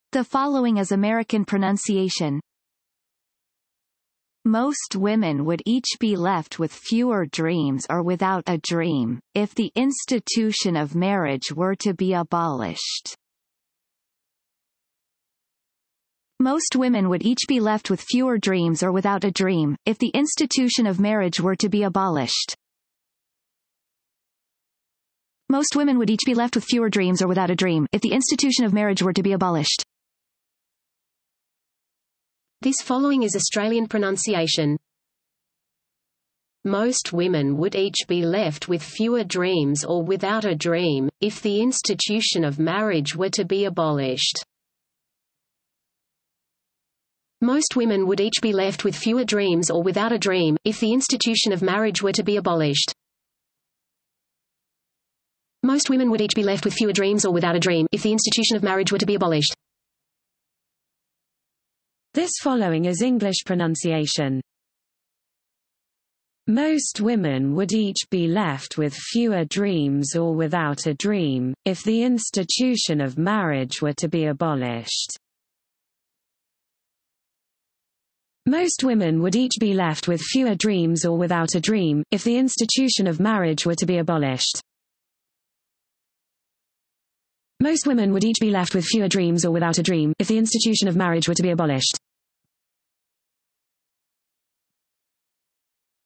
Most women would each be left with fewer dreams or without a dream, if the institution of marriage w
# EveryDay Saying Aphorism Quotes in American、Australian、English pronunciation # Most women would each be left with fewer dreams or without a dream, if the institution of marriage were to be abolished. - Mokokoma Mokhonoana
saying aphorism in different english pronunciation
 Learn english # English pronunciation practice
 English Pronunciation Training Improve Your Accent Speak Clearly